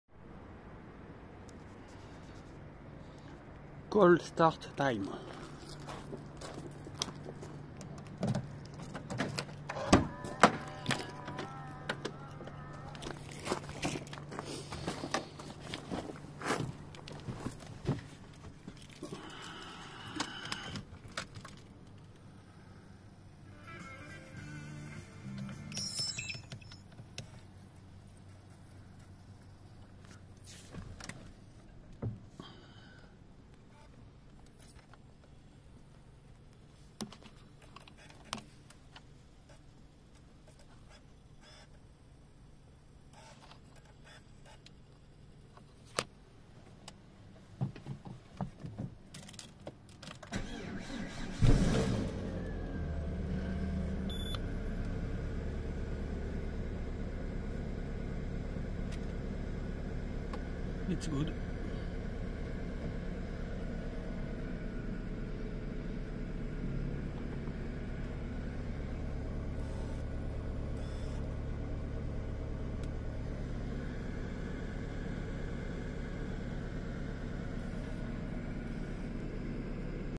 E85 COLD START Peugeot 407 2.2i 16V EW12J4 engine éthanol motor démarrage à froid
COLD START January Peugeot  PSA 407 2.2i 16V with E85 , year 2004
25/01/2023 , 166000km
Engine type EW12J4
Dosage with 96% of E85
Oil 5W40
Battery Hankook 68Ah New
E85 Intermarché